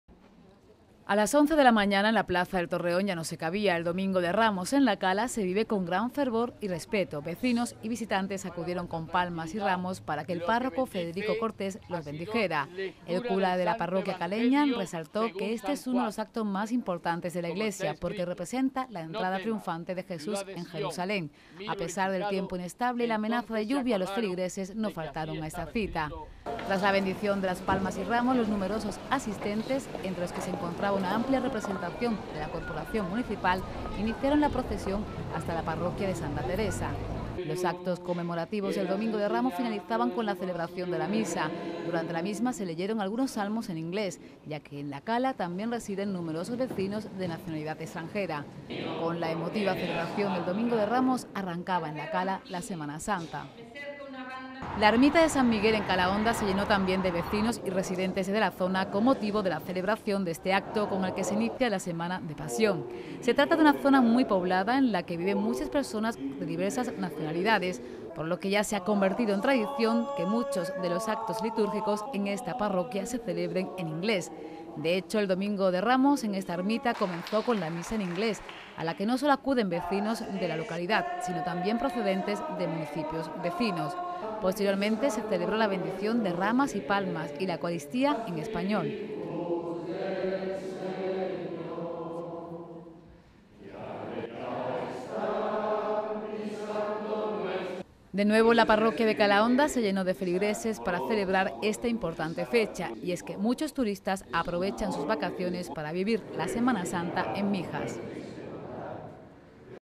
Domingo de Ramos en La Cala y Calahonda
Fe e ilusión también acompañaron en la celebración en La Cala y Calahonda, de uno de los actos más importantes de la iglesia, la escenificación de la entrada de Jesús a Jerusalén. En ambos casos, hubo lectura de la misa en inglés, debido al elevado número de vecinos extranjeros que residen en la zona